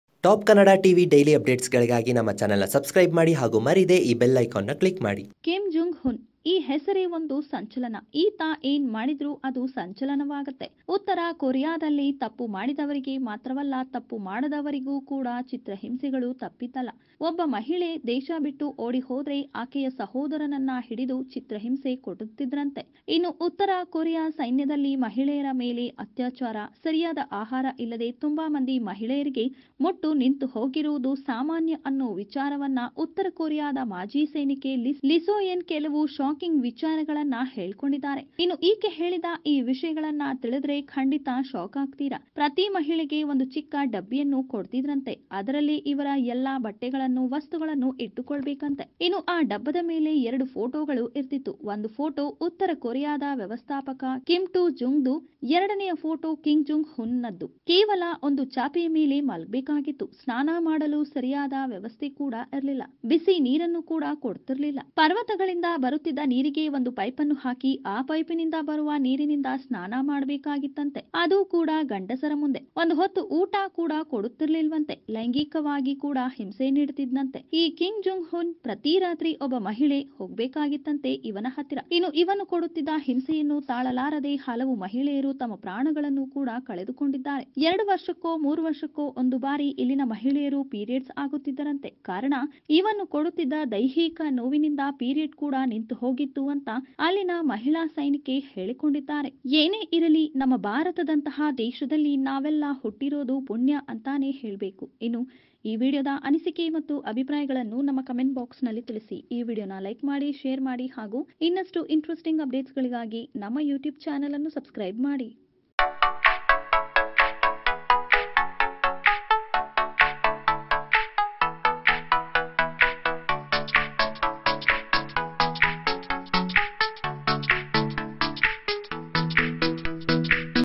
ಮಹಿಳೆಯರಿಗೆ ಪಿರಿಯಡ್ಸ್ ಬರದಹಾಗೆ ಮಾಡಲು ಕಿಮ್ ಎಂಥಹ ಕೆಲಸ ಮಾಡಿದ್ದಾನೆ ತಿಳಿದರೆ ಕಣ್ಣಲ್ಲಿ ನೀರು ಬರುತ್ತೆ ..!
Watch ಮಹಿಳೆಯರಿಗೆ ಪಿರಿಯಡ್ಸ್ ಬರದಹಾಗೆ ಮಾಡಲು ಕಿಮ್ ಎಂಥಹ ಕೆಲಸ ಮಾಡಿದ್ದಾನೆ ತಿಳಿದರೆ ಕಣ್ಣಲ್ಲಿ ನೀರು ಬರುತ್ತೆ ..!

Kannada Short films, Darshan, Sudeep, Yash, Shivarajkumar, Puneethrajkumar Sandalwood updates,news,gossips and interviews